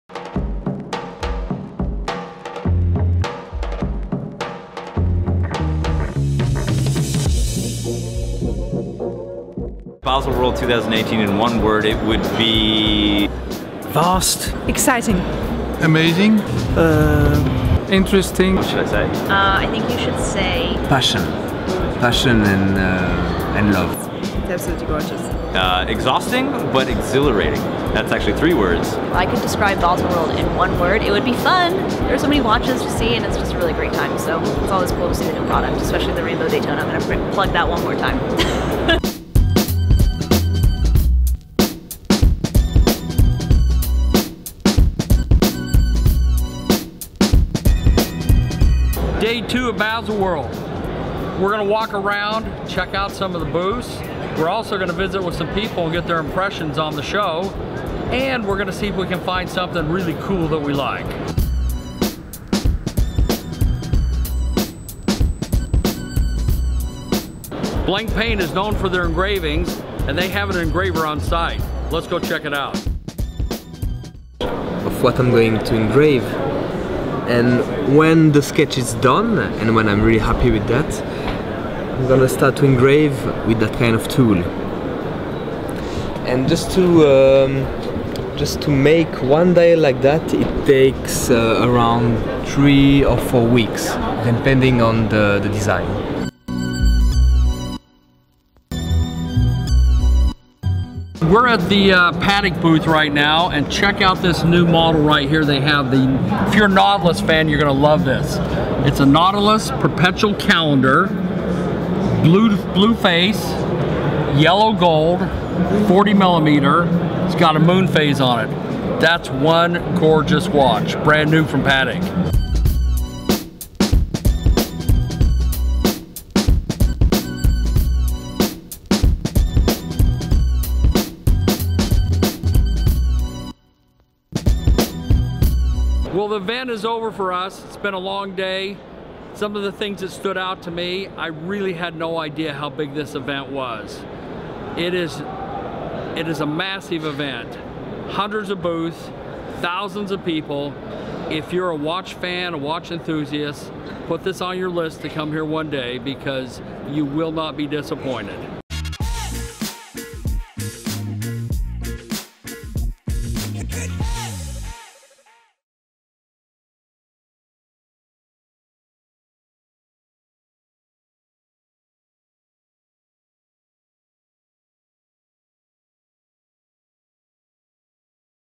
Special Guests at Baselworld 2018 - Episode III
Final episode with our special guests at Baselworld 2018 and we think that they had a really good time being surrounded by that many watches and watch lovers!!!